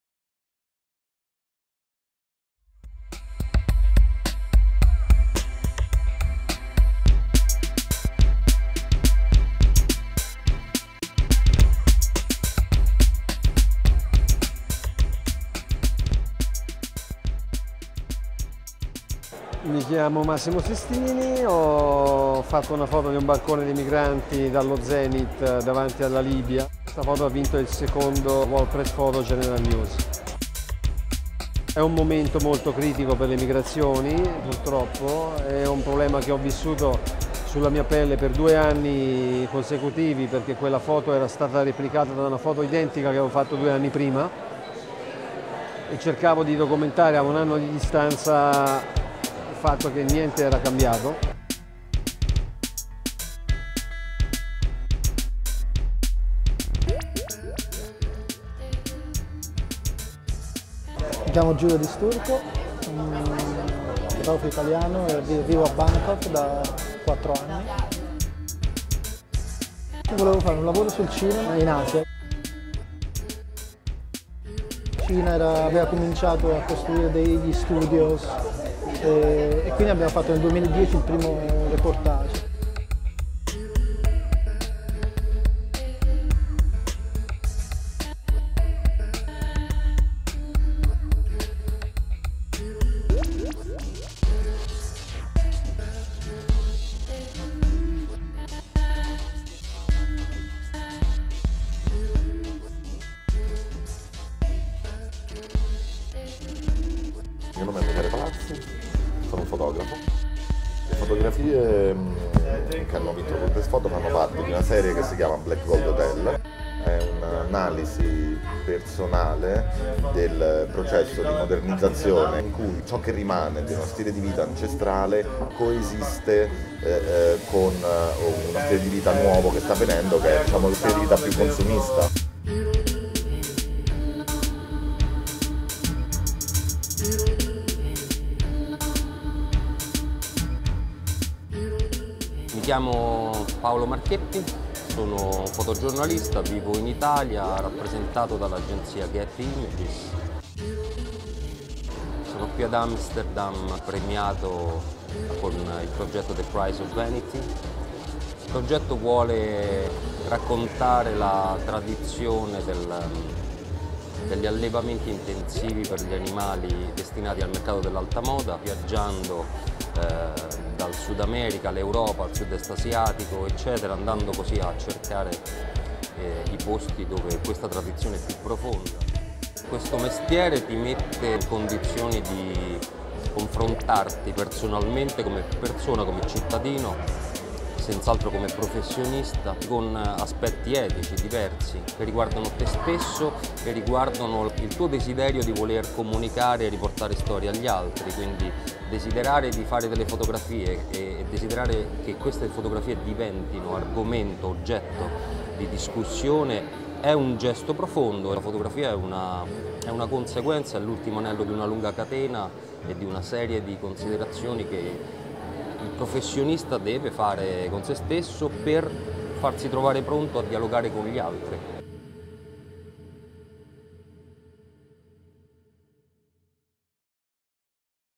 [ITA] World Press Photo 2015, +31mag meets three italian award winners - VIDEOSTORY
Three Italian award winners @ World Press Photo 2015, talk about their projects and their work as photojournalists. Amsterdam May 2015